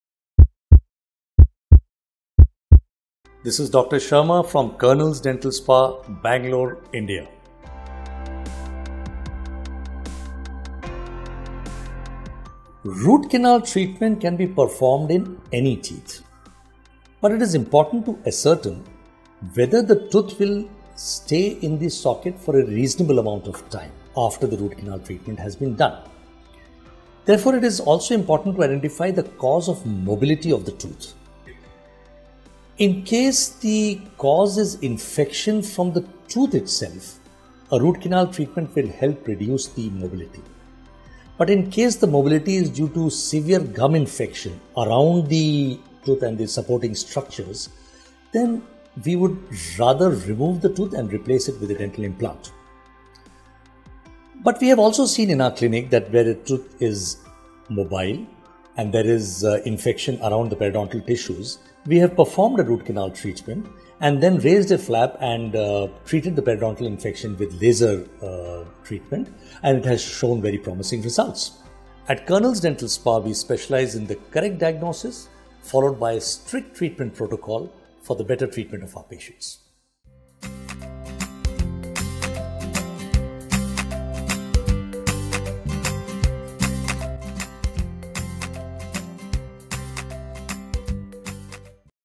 Can RCT be performed in mobile teeth? - Dr. Manesh Chandra Sharma
Root canal treatment can be performed in any teeth but it is important to ascertain whether the tooth will stay in the socket for a reasonable amount of time after the root canal treatment has been done. There for it is also important to know the cause for the mobility of the tooth. In case the cause is from the infection of the tooth itself, a root canal treatment will help reduce the mobility. But in case the mobility is due to severe gum infection around the tooth and the supporting structures, then we would rather remove the tooth and replace it with a dental implant. But there are cases where the tooth is  mobile an there is infection around the  periodontal tissues and we have performed a root canal treatment and raised a flap and treated the periodontal infection with laser treatment and has shown very promising results.